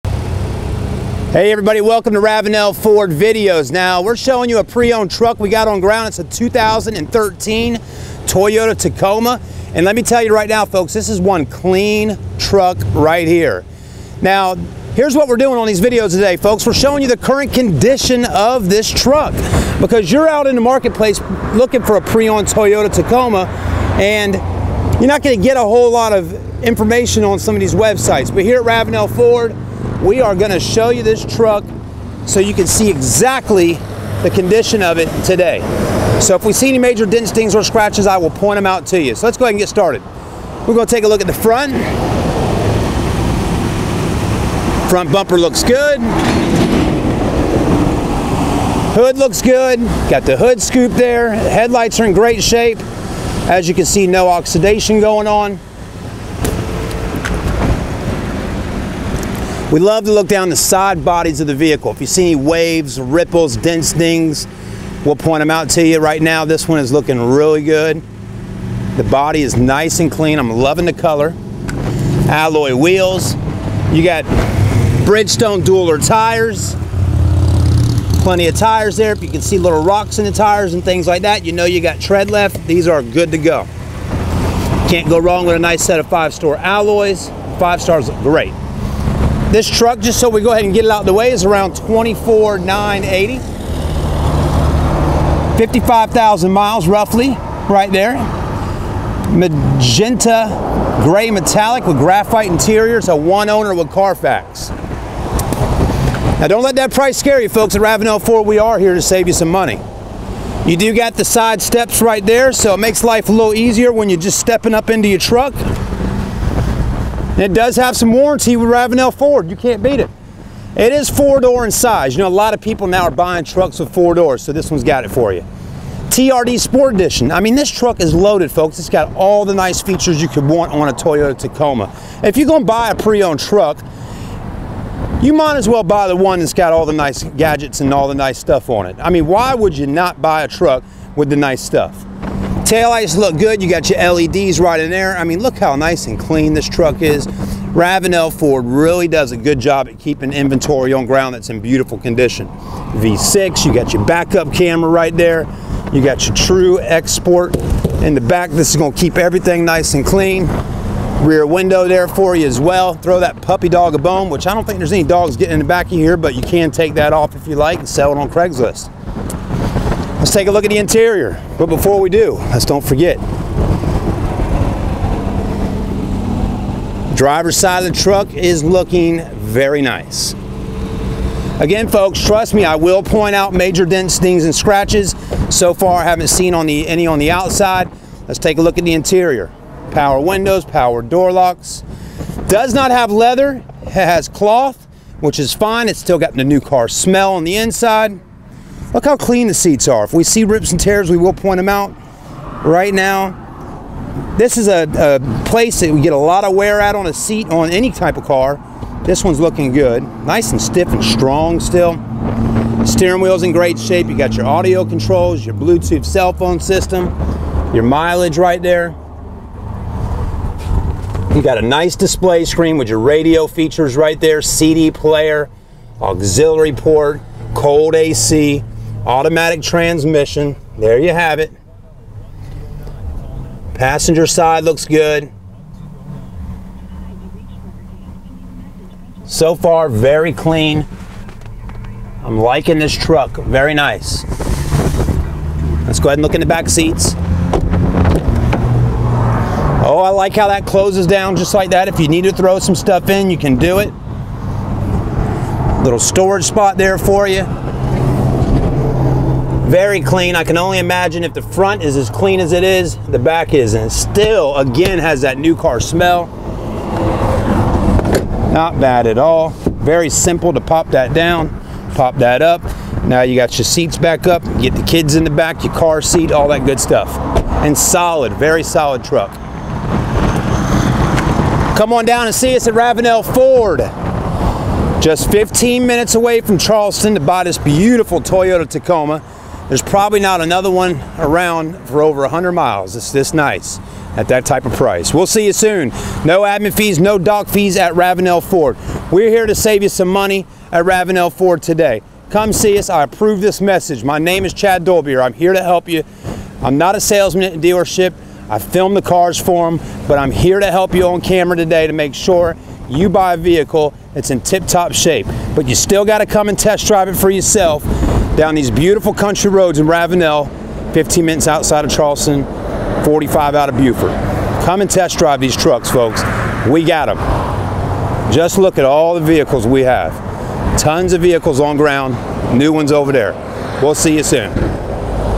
2013 Toyota Tacoma Prerunner V6 with TRD Package - FULL WALKAROUND @ Ravenel Ford
If interested in more info about this vehicle make sure to contact the dealership!